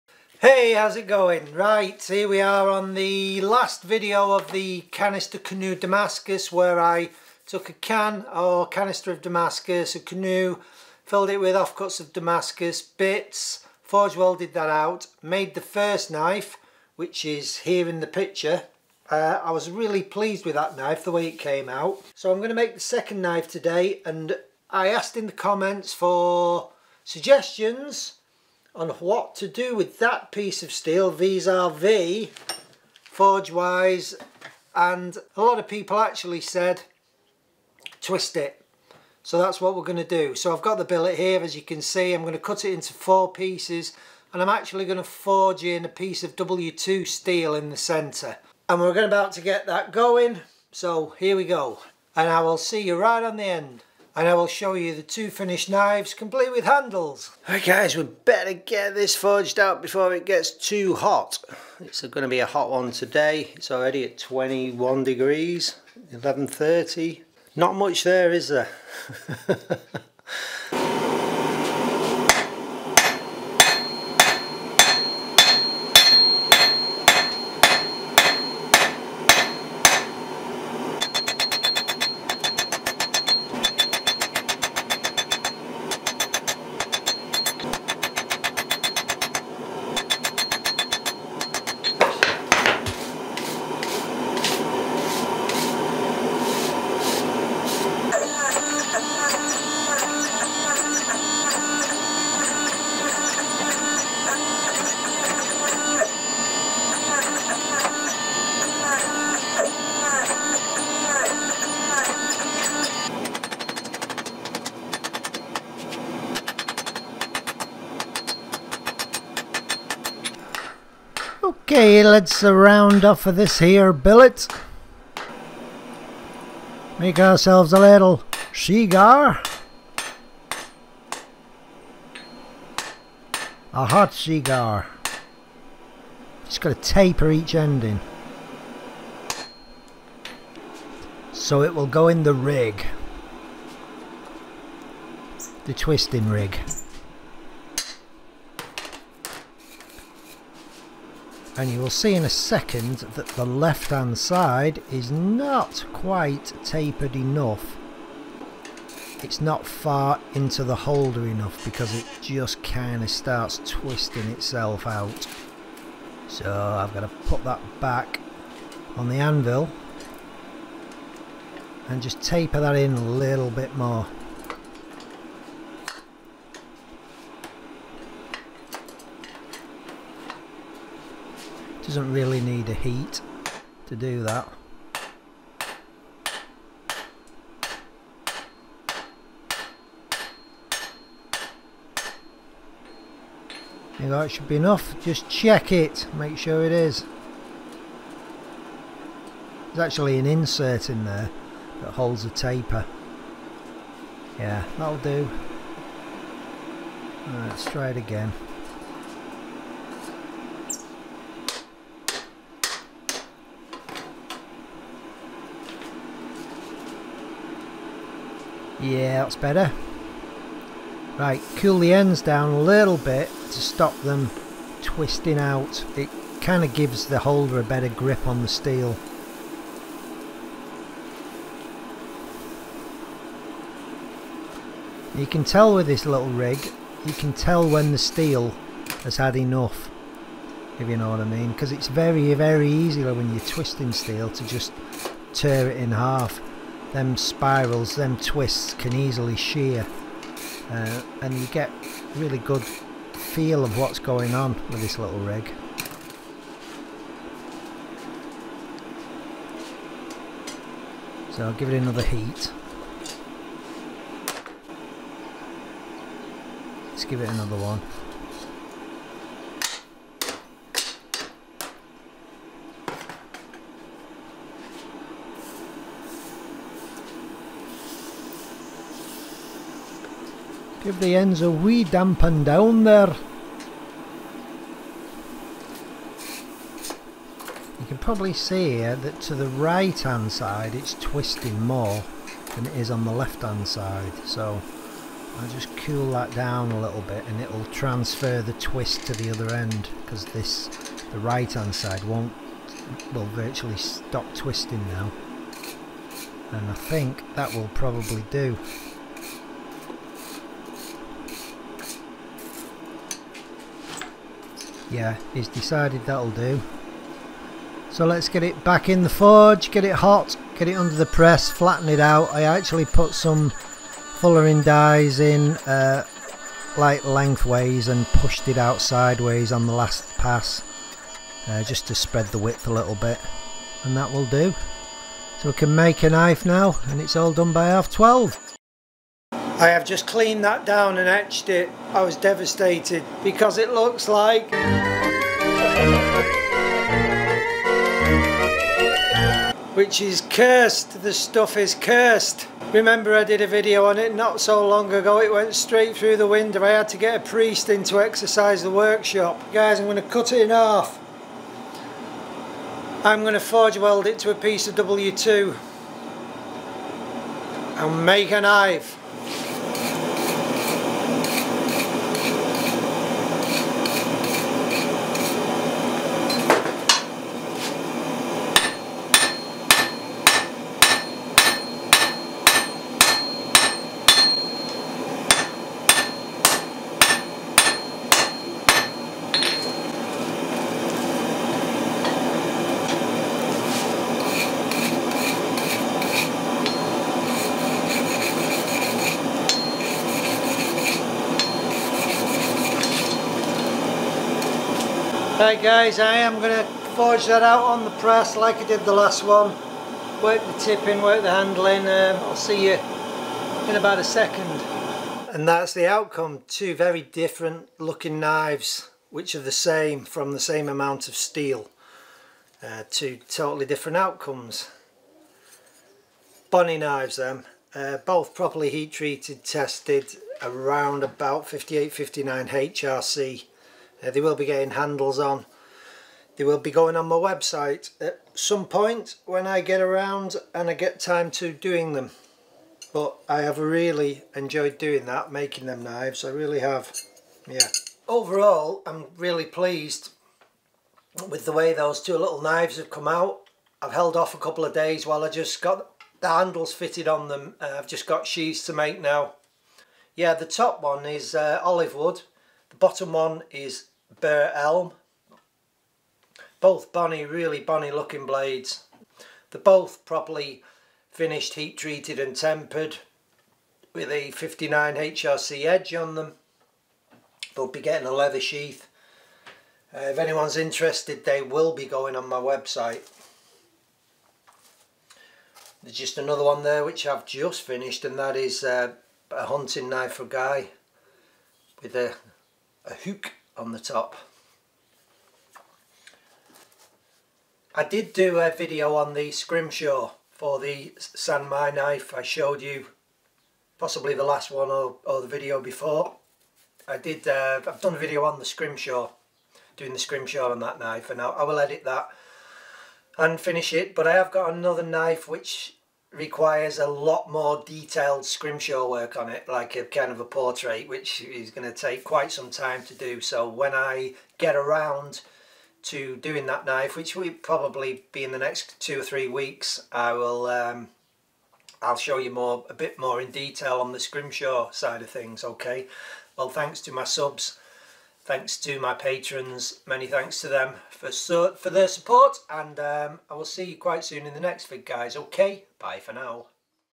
canoe damascus making the 2nd knife
Finishing off the billet making the second knife by twisting the damascus